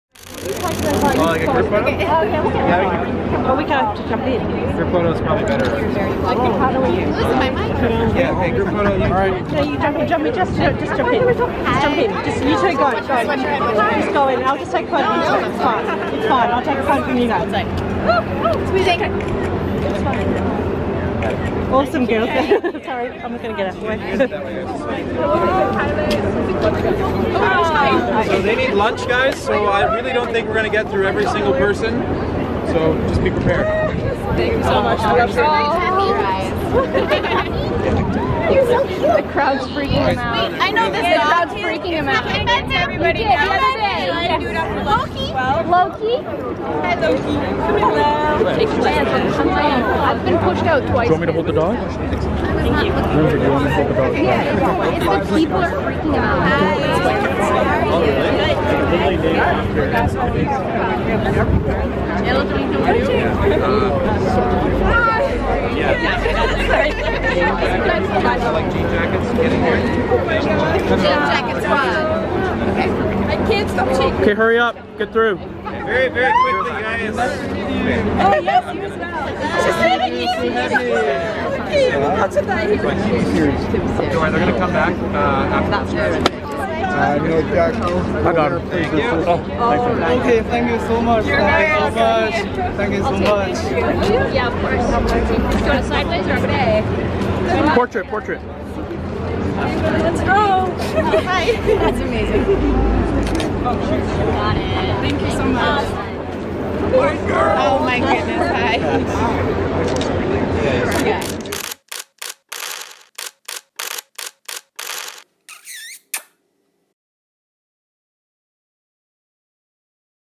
Supergirl Season 3 - Chyler Leigh and Floriana Lima Meet Fans in Vancouver!
Chyler Leigh and Floriana Lima meet Supergirl Fans in Vancouver!

Chyler Leigh and Floriana Lima meet Supergirl Fans in Vancouver! Supergirl season 3 is back filming in Vancouver, British Columbia. Two of the main stars Chyler Leigh and Floriana Lima were seen on set as they posed for pictures with fans at the set in downtown Vancouver.

You can also visit our Celebrity WotNot website for more great celebrity coverage